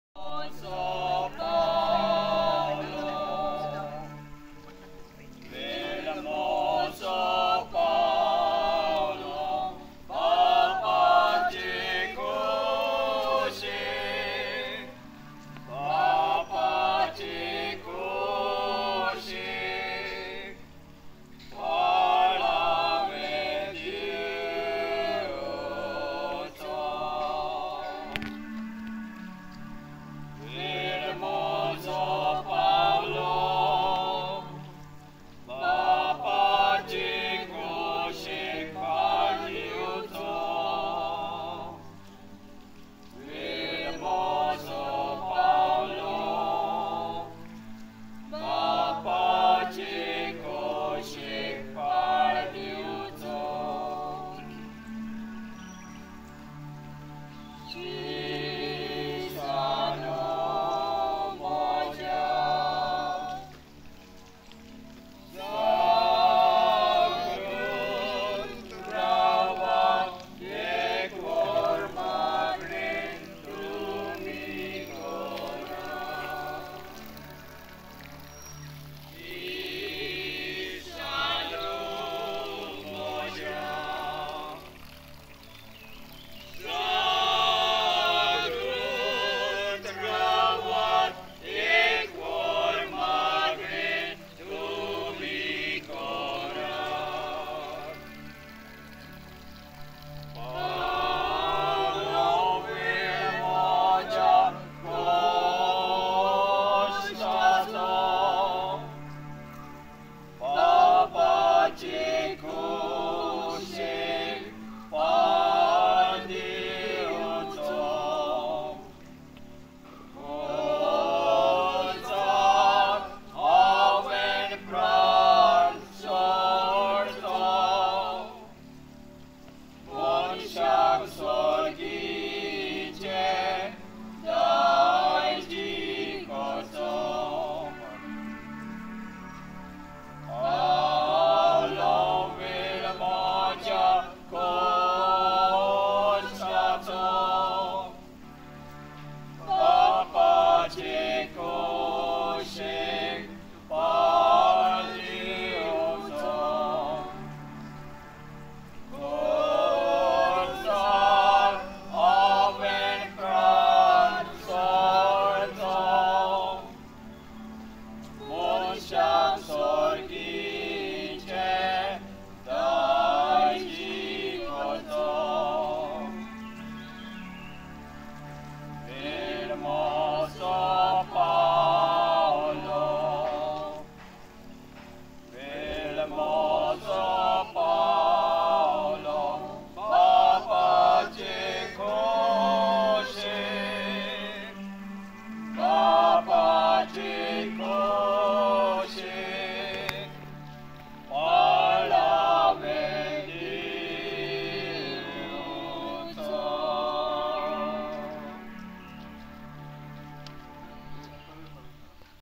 Motets at Salmona (7/8)
Organised by Simon Fernandes, Orland Vaz and team. An old tradition of religious music revived in a scenic, sylvan spot. March end 2023. Recorded by Frederick Noronha (FN).